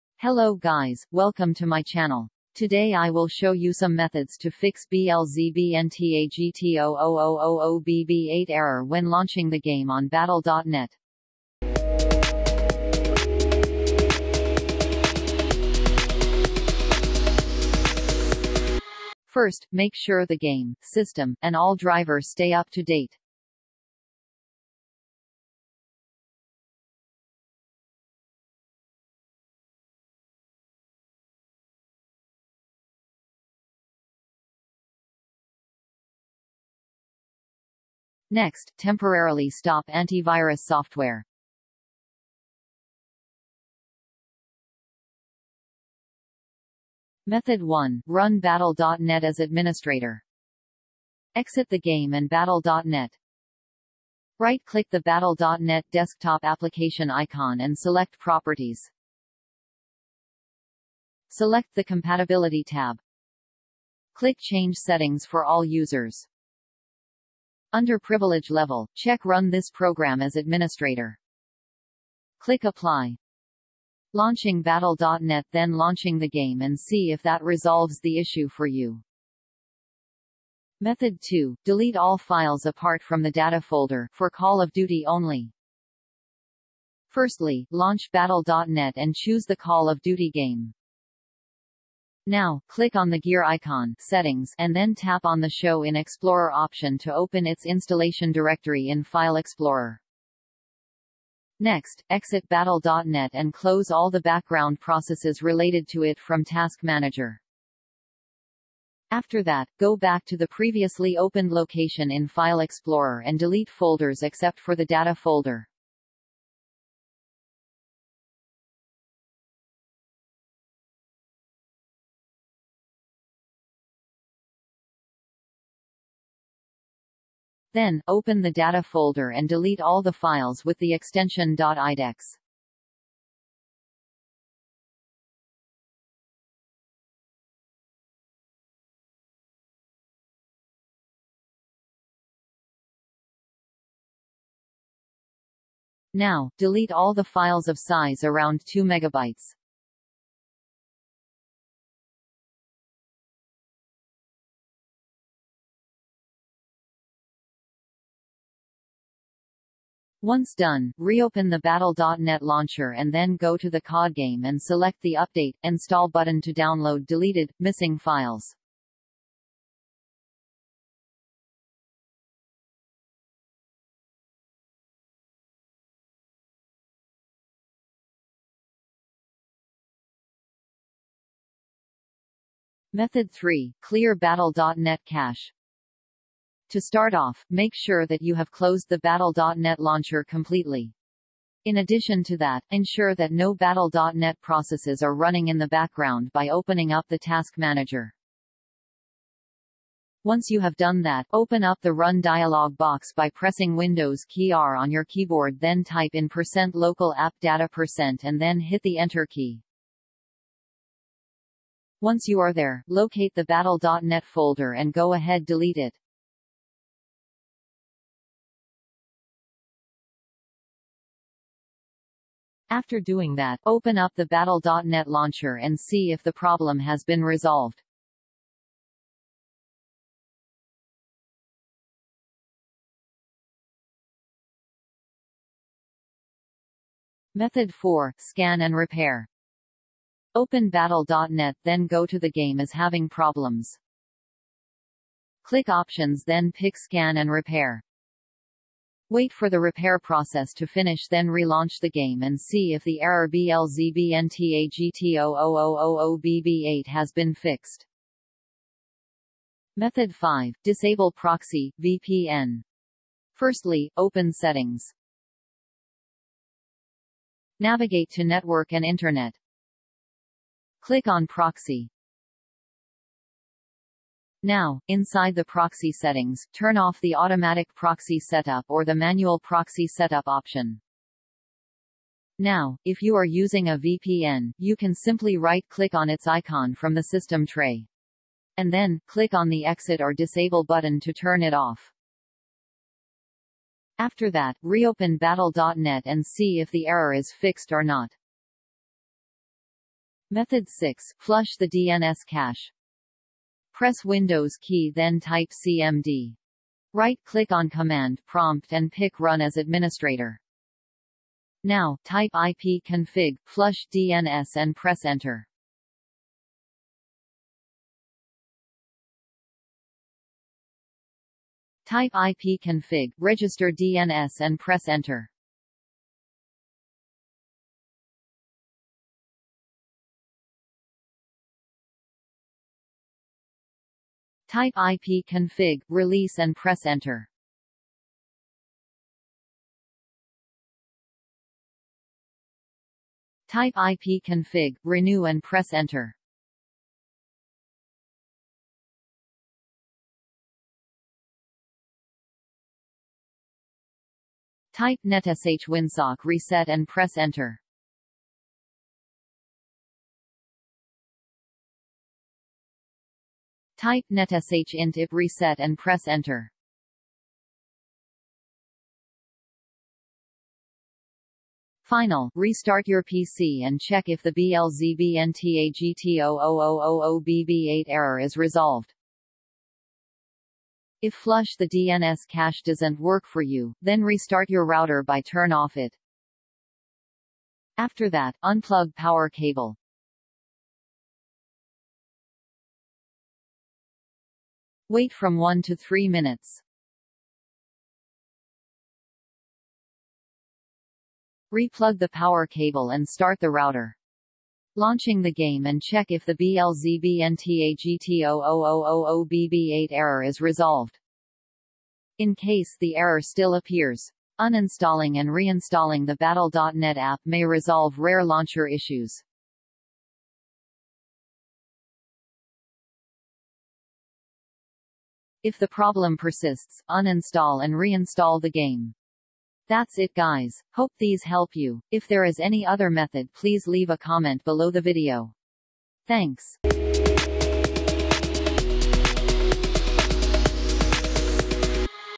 8 Methods To Fix BLZBNTAGT00000BB8 error when launching the game on Battle.net
Battle.net Whoops! Looks like something broke. Give it another shot.
Error Code BLZBNTAGT00000BB8